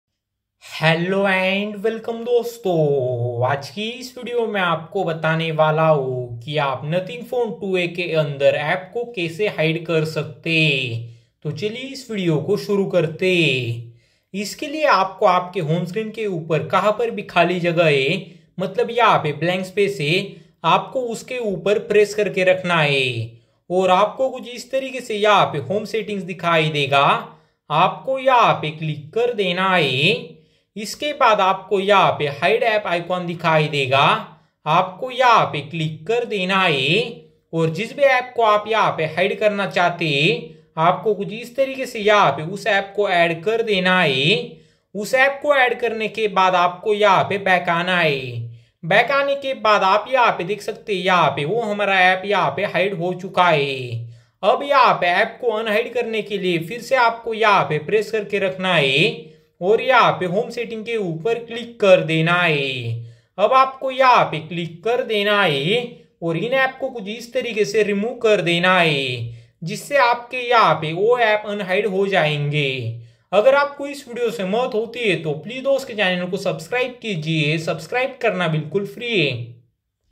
how to hide app in nothing phone 2a | nothing phone me apps kaise hide kare
This video is about how to hide apps in nothing phone 2a app ko kaise hide kare nothing